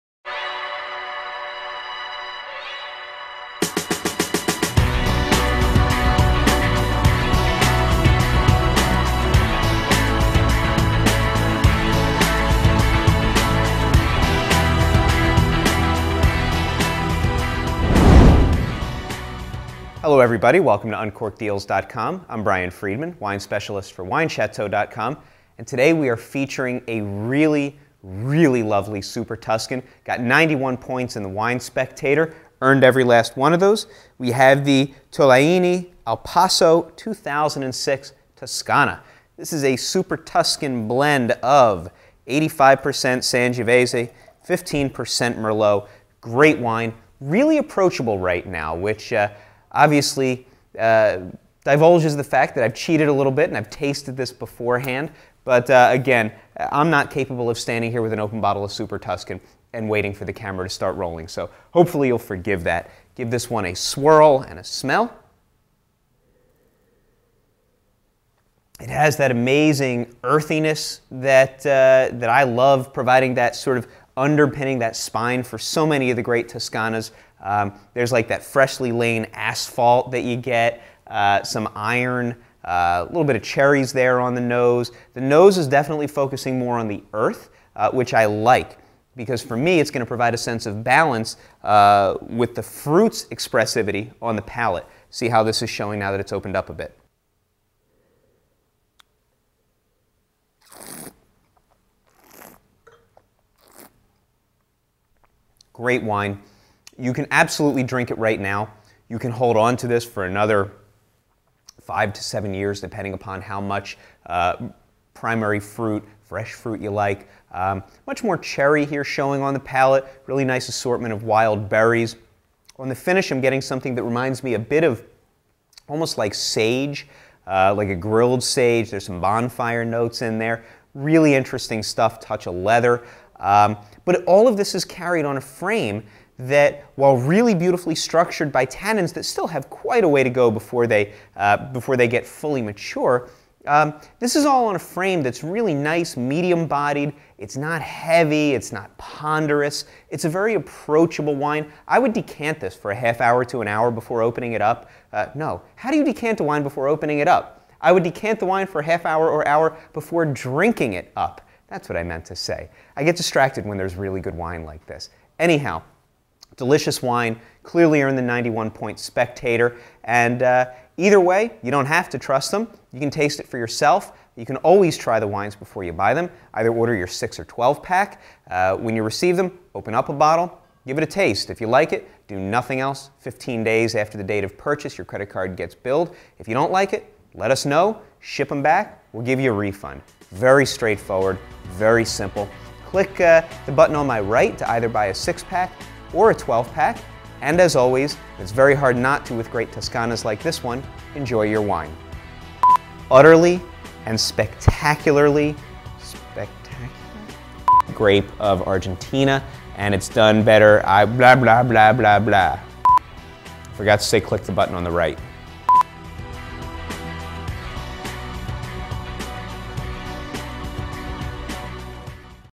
UncorkDeals.com - Tolaini Al Passo Toscana
Watch as Brian Freedman, the wine specialist for Wine Chateau, uncorks his experiences on this wine on Uncork Deals.